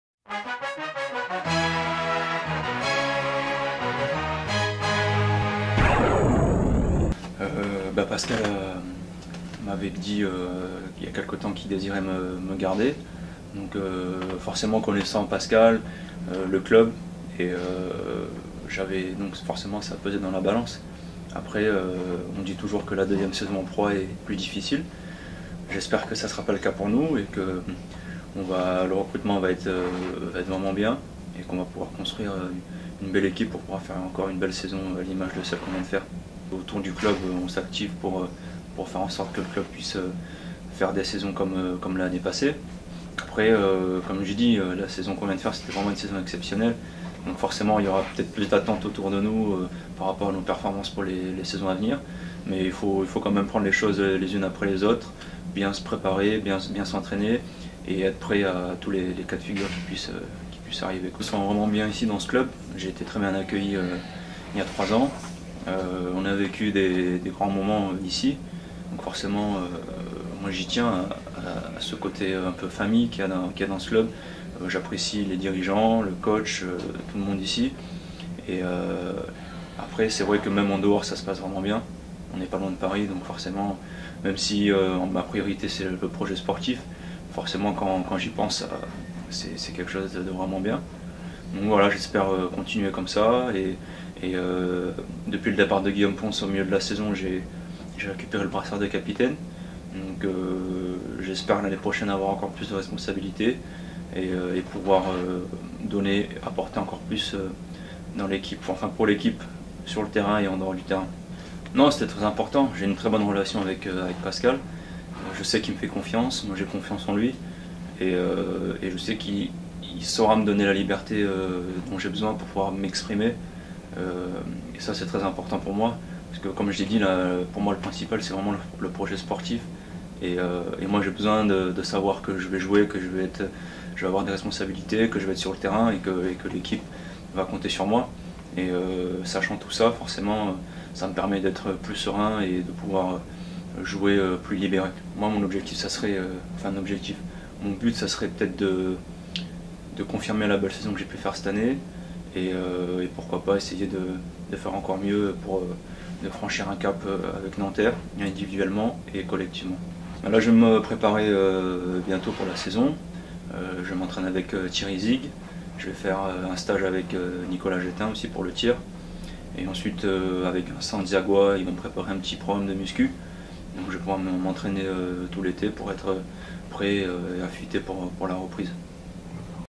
JSFN TV: Xavier Corosine prolonge avec Nanterre
L'arrière et shooteur fou de la JSF Nanterre, Xavier Corosine rempile pour deux saisons avec le club Nanterrien.